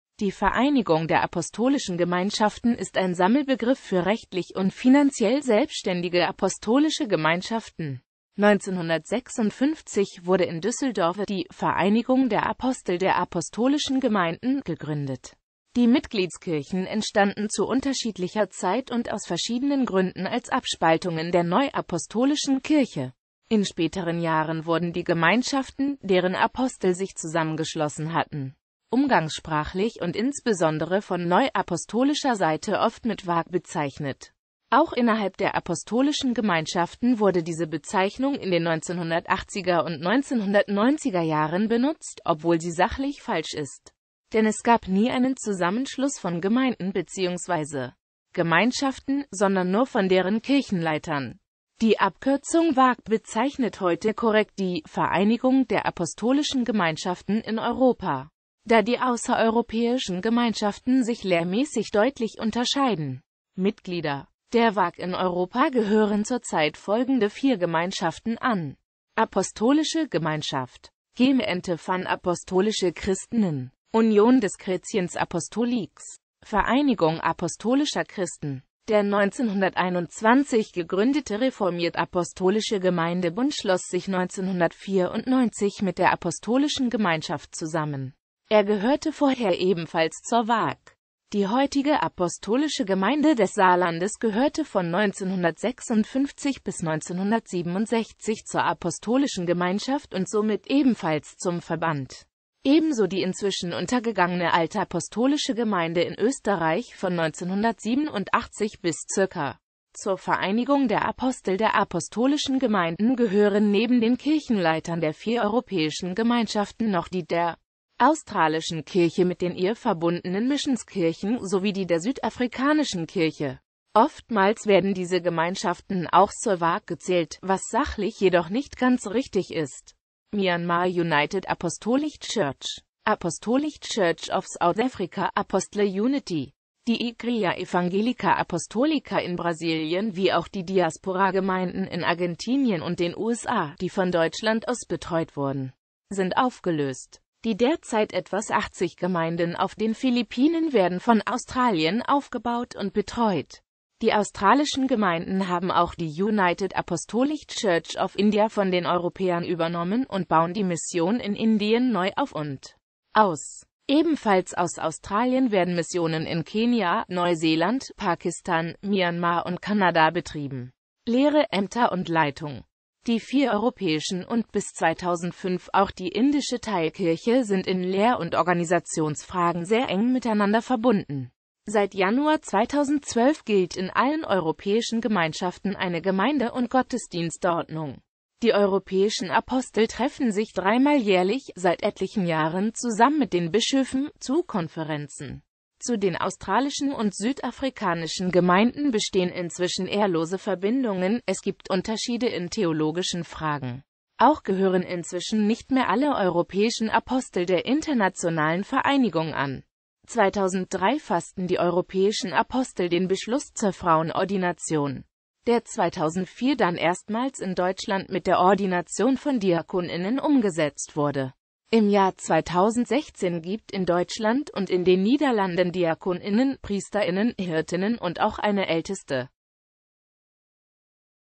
Vereinigung Apostolischer Gemeinden
Vielen Dank für Ihre Unterstützung:

Die Vereinigung der Apostolischen Gemeinschaften (abgekürzt VAG, oder englisch United Apostolic Church) ist ein Sammelbegriff für rechtlich und finanziell selbständige apostolische Gemeinschaften.1956 wurde in Düsseldorf die „Vereinigung der Apostel der Apostolischen Gemeinden“ gegründet.Die Mitgliedskirchen entstanden zu unterschiedlicher Zeit und aus verschiedenen Gründen als Abspaltungen der Neuapostolischen Kirche.In späteren Jahren wurden die Gemeinschaften, deren Apostel sich zusammengeschlossen hatten, umgangssprachlich und insbesondere von neuapostolischer Seite oft mit VAG bezeichnet.

Bild-Copyright-Informationen
Urheber Info: derivative work: DenkerChris (talk) Logo-uac.jpg: unknown
Lizenz: Public domain
Urheber: